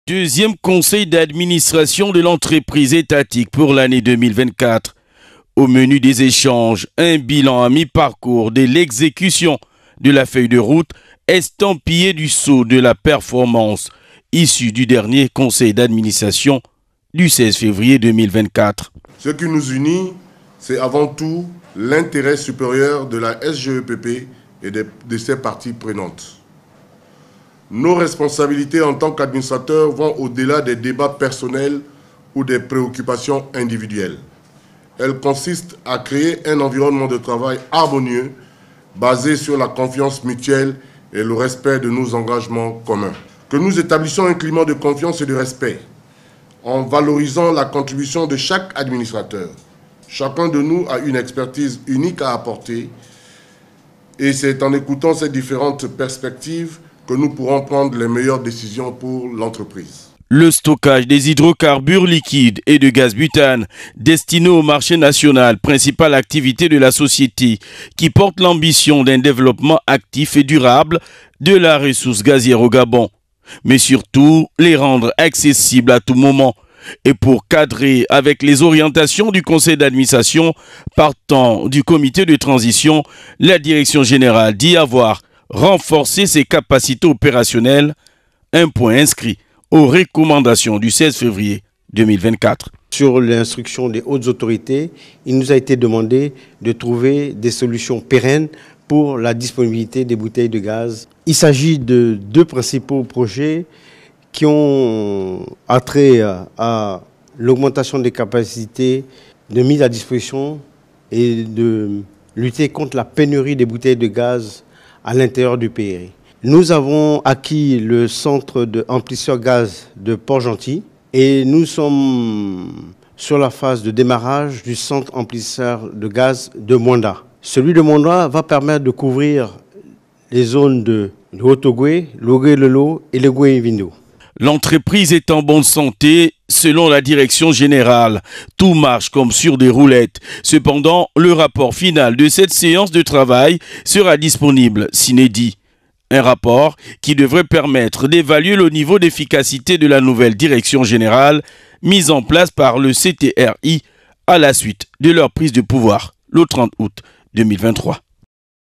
𝐆𝐀𝐁𝐎𝐍 : 𝐋𝐀 𝐒𝐆𝐄𝐏𝐏 𝐓𝐈𝐄𝐍𝐓 𝐒𝐎𝐍 𝐃𝐄𝐔𝐗𝐈𝐄̀𝐌𝐄 𝐂𝐎𝐍𝐒𝐄𝐈𝐋 𝐃'𝐀𝐃𝐌𝐈𝐍𝐈𝐒𝐓𝐑𝐀𝐓𝐈𝐎𝐍 𝟐𝟎𝟐𝟒
Sous la houlette du général de brigade , Florient PANGOU MBEMBOU, la société gabonaise d'entreposage des produits pétroliers SGEPP, a tenu son deuxième conseil d'administration après celui de février dernier.
Il a été question de faire un bilan de l’avancement de la feuille de route de la direction générale.